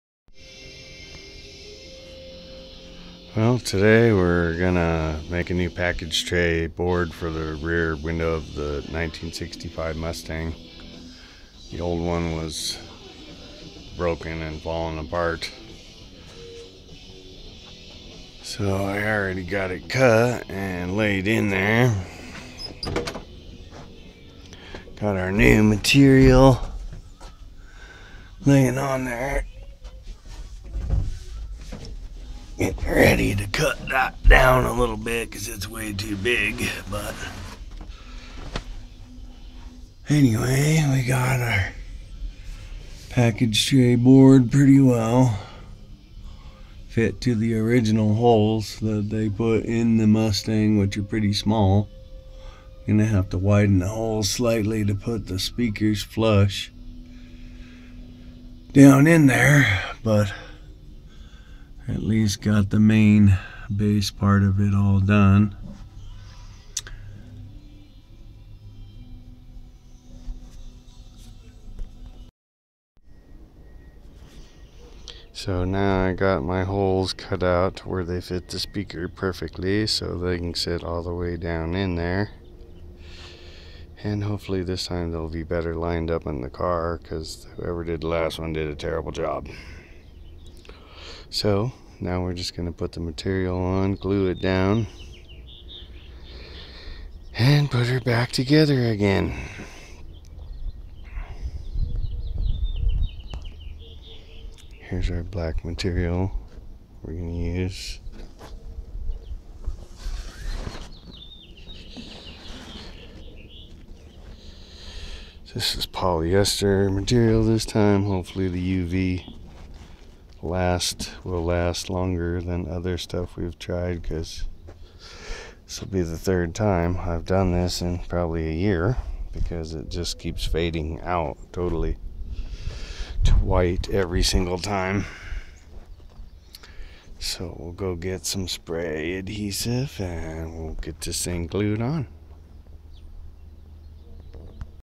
My 1965 mustang
Building new package tray with new uphostery